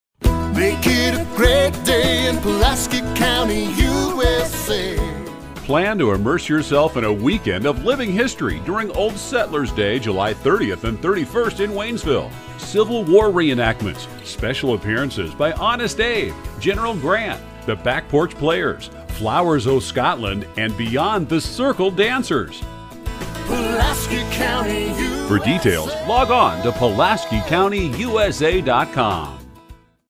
Old Settlers Day & Civil War Reenactments
Plan to immerse yourself in a weekend of living history during Old Settlers Day July 30 & 31st in Waynesville! Civil War Reenactments, special appearances by Honest Abe, General Grant, The Back Porch Players, Flowers O'Scotland, and Beyond The Circle Dancers!